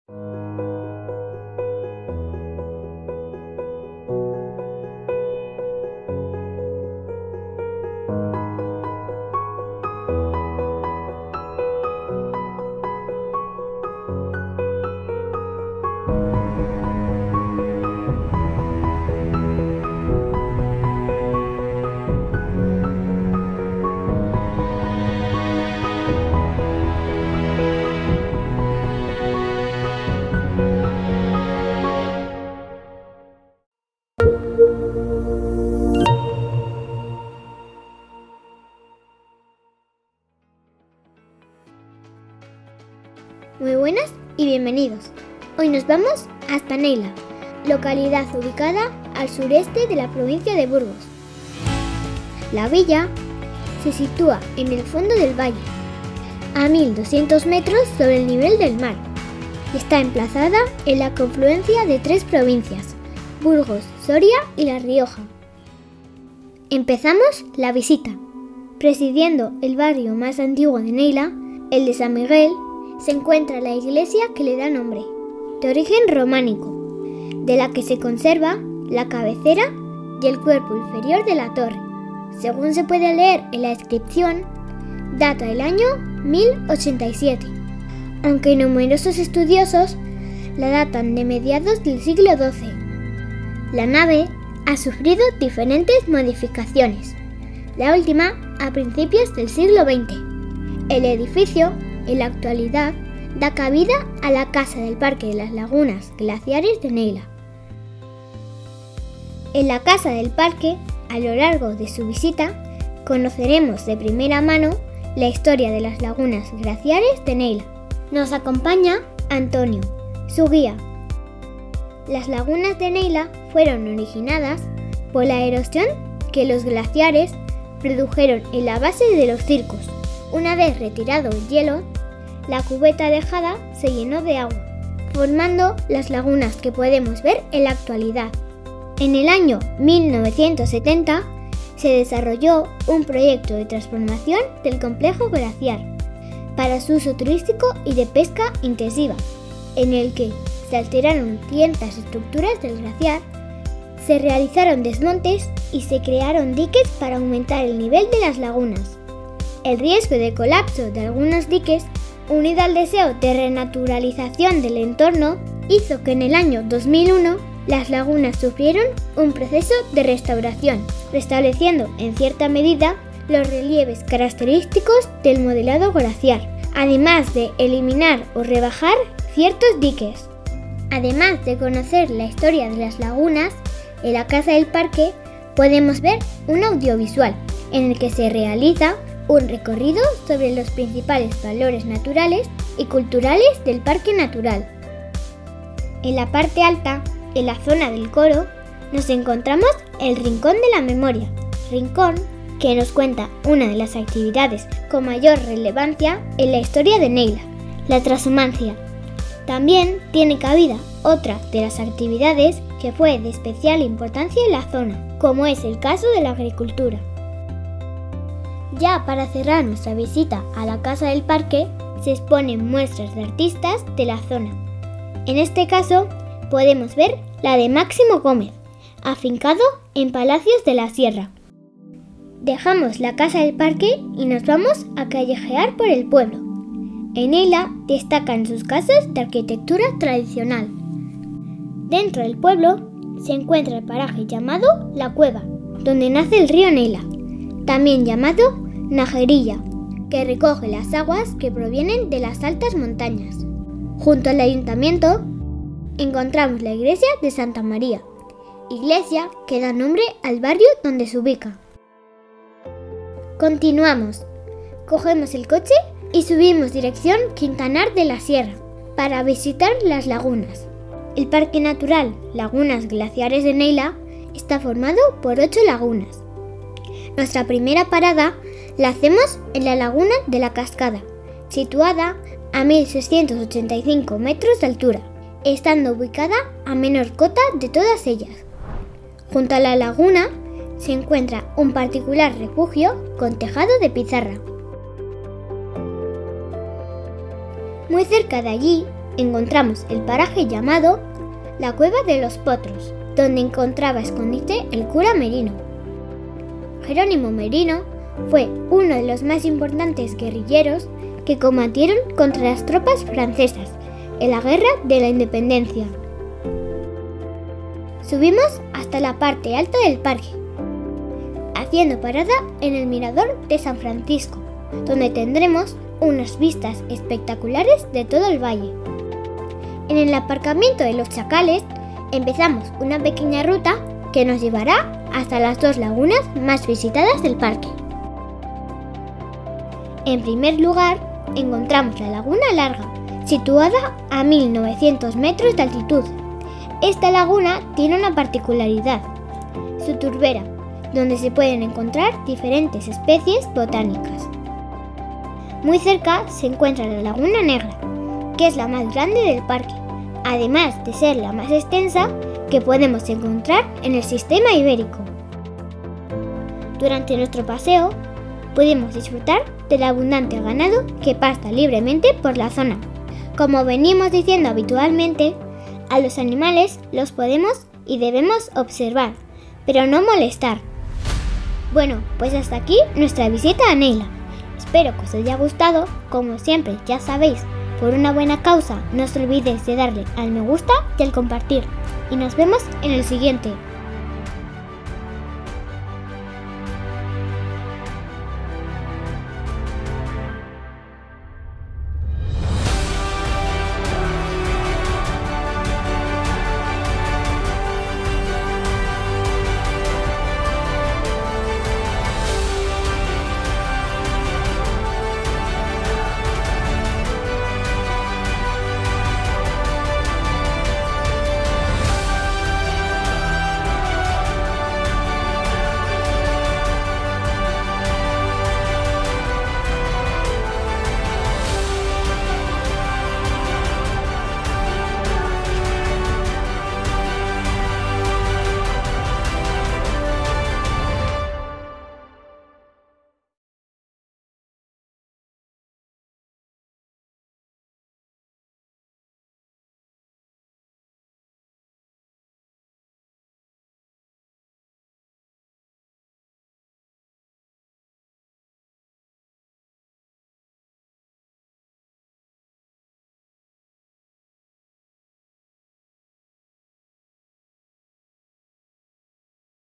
Neila, Lagunas Glaciares de Neila
En el video de hoy nos vamos hasta la zona sureste de la provincia de Burgos, en concreto a la localidad de Neila, villa que da nombre al Parque Natural Lagunas Glaciares de Neila; como no podía ser de otro modo, empezamos nuestra visita en la casa del parque, ubicada en Neila, para conocer de primera mano la historia de las lagunas y su entorno natural, después callejearemos por el pueblo de Neila y por último nos subiremos a conocer, in situ, las diferentes lagunas que forman el parque natural.
Agradecer la colaboración de Marino y Antonio a la hora de realizar la grabación, también que tenemos que dar las gracias a la dirección del parque natural lagunas glaciares de Neila por la concesión de los permisos necesarios.